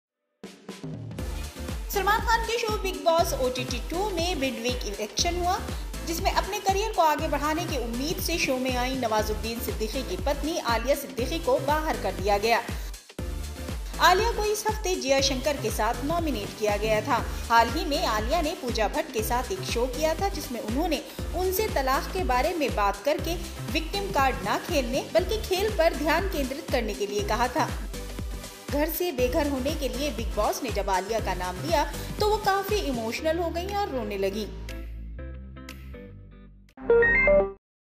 मिड वीक एविक्शन में बाहर हुईं नवाजुद्दीन की पत्नी आलिया सिद्दीकी | Alia Siddiqui
सलमान खान के शो 'बिग बॉस ओटीटी 2' में मिड वीक एविक्शन हुआ. जिसमें अपने करियर को आगे बढ़ाने की उम्मीद से शो में आईं नवाजुद्दीन सिद्दीकी की पत्नी आलिया सिद्दीकी को बाहर कर दिया गया. आलिया को इस हफ्ते जिया शंकर के साथ नॉमिनेट किया गया था.